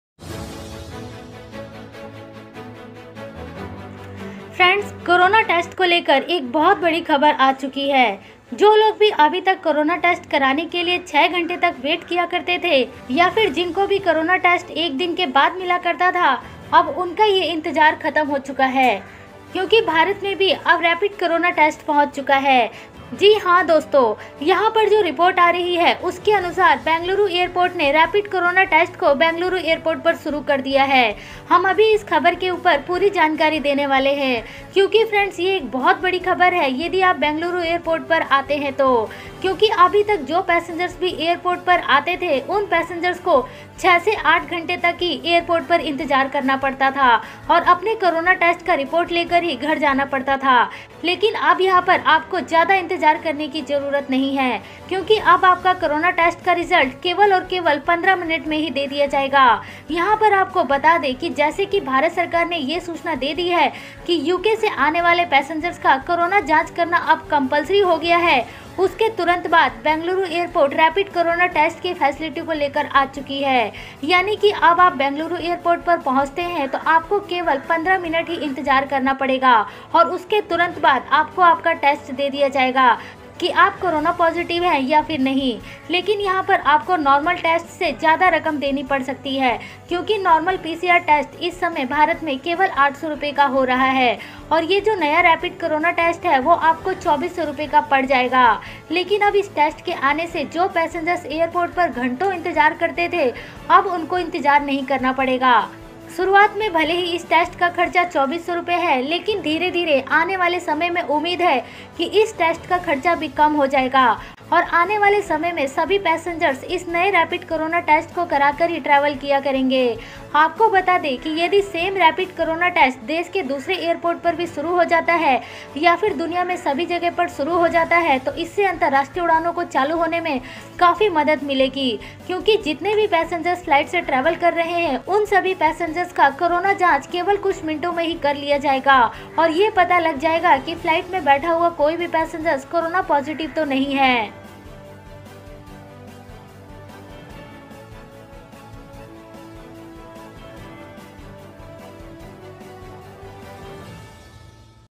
Now in India, Corona Test in 15 Minutes. Good News for International Passengers.
New Changes in Air Bubbles Agreement
shorturl.at/wAKRZ